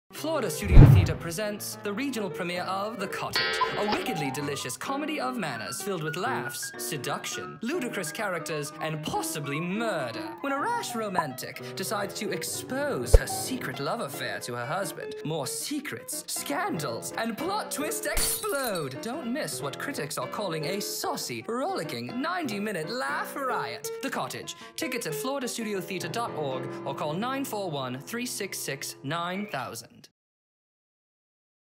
FST Mainstage | The Cottage
The Cottage
By Sandy Rustin
July 31-August 24, 2019 (Gompertz Theatre)

REGIONAL PREMIERE. Set in the English countryside in 1923, this sexy comedy of love and betrayal unfolds when Sylvia Van Kipness decides to expose her secret love affair to her husband. The true meanings of love and marriage are called into question as a surprising, hilarious web of secrets unravels in this ridiculous—potentially murderous—romantic comedy.